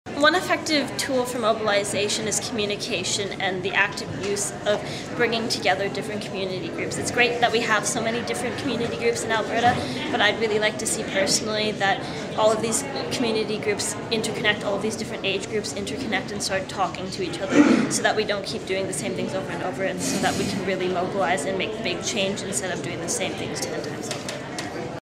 Make Shift Happen: Effective Ways to Mobilize 4
An idea from PIA's 2012 Annual Advocacy Conference, Make Shift Happen: Mobilizing the Power of the People.